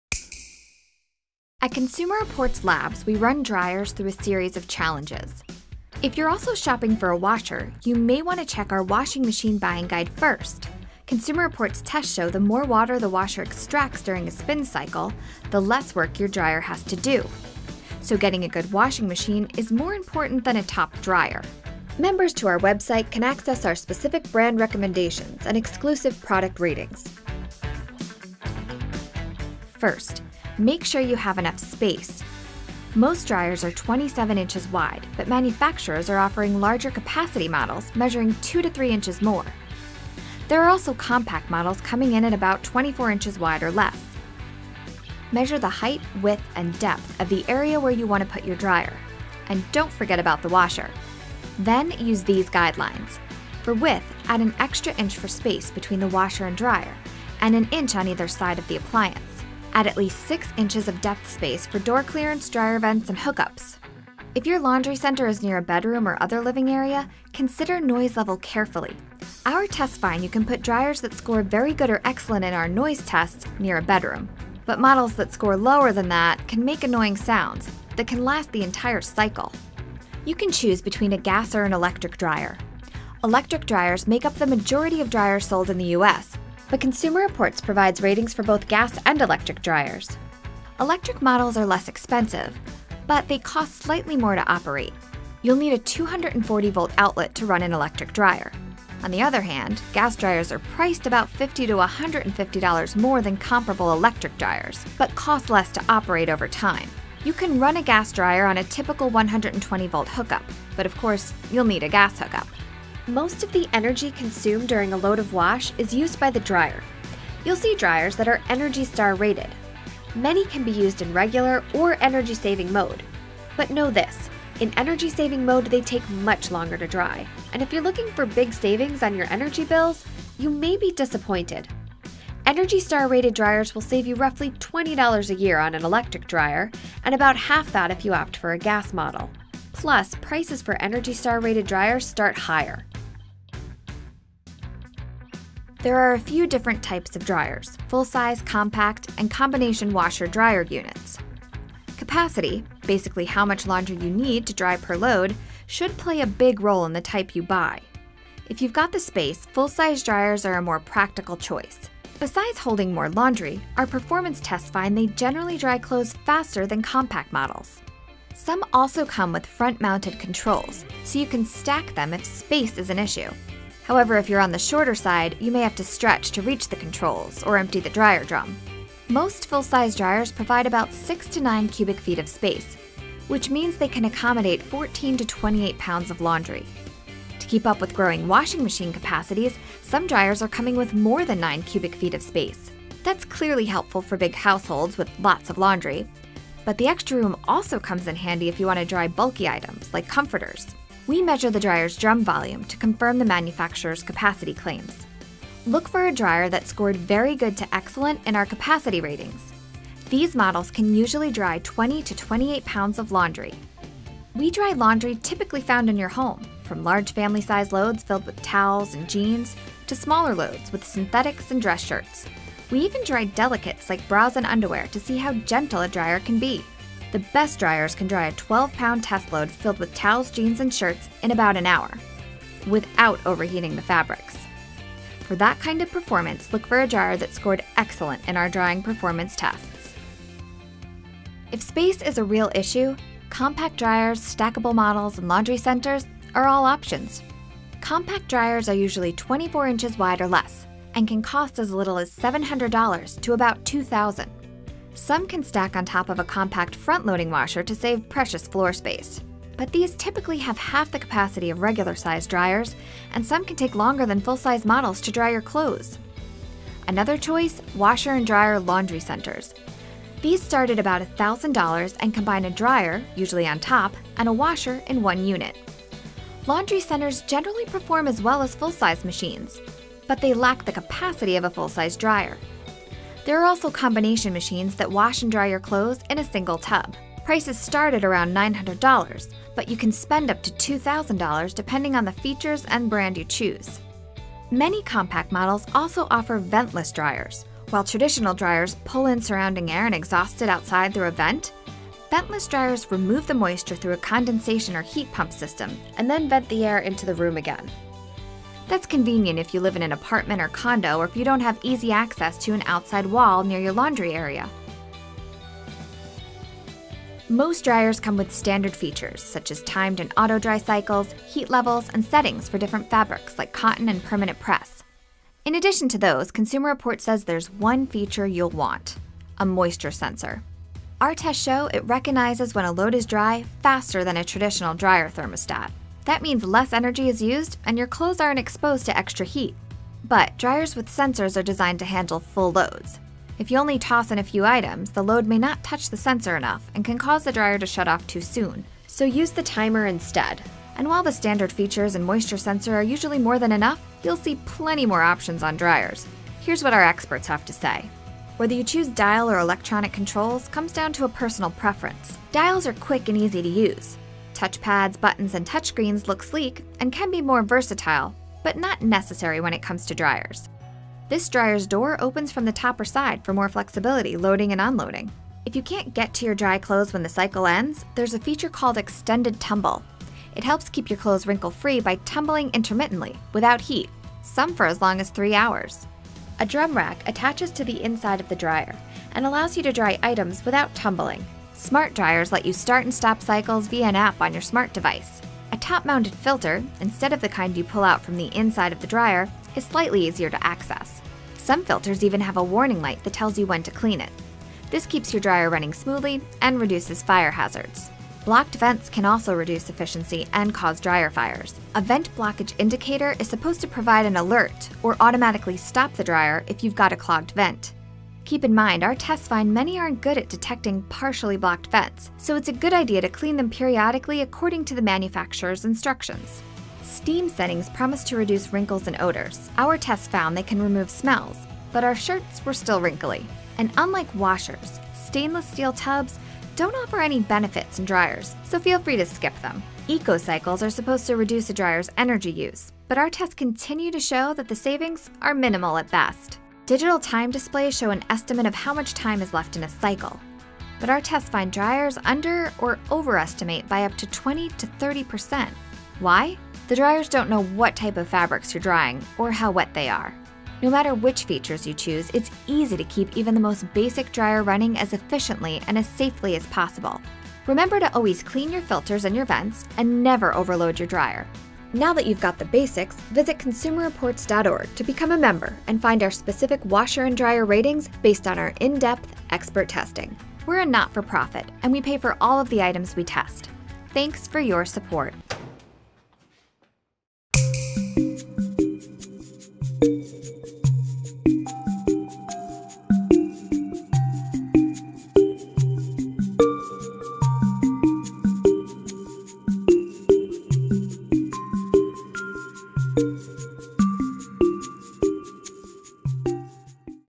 Clothes Dryer Buying Guide | Consumer Reports
Consumer Reports experts walk you through which type of dryer is best for your home and the features you actually need.